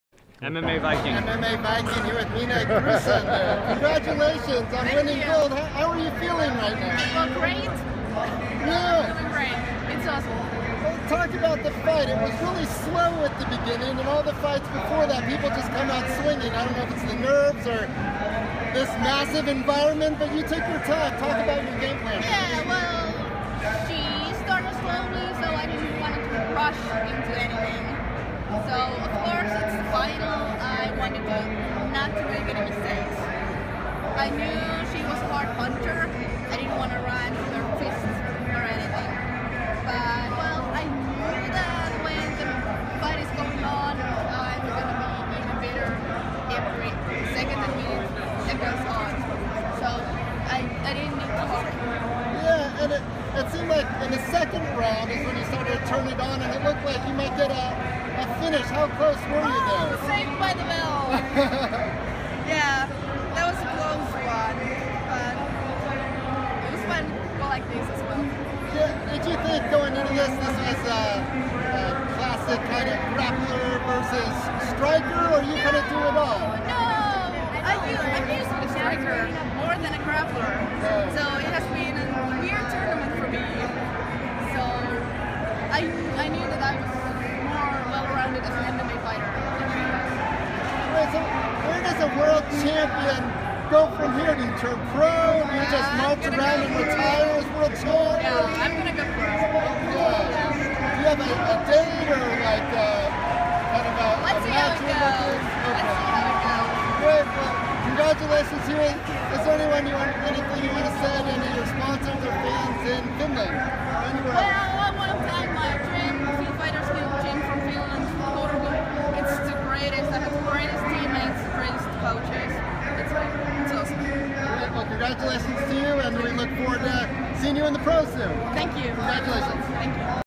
Minna Grusandar Wins Gold - Post Fight Interview
Finland’s Minna Grusander talks to mmaviking.com in Las Vegas after IMMAF win.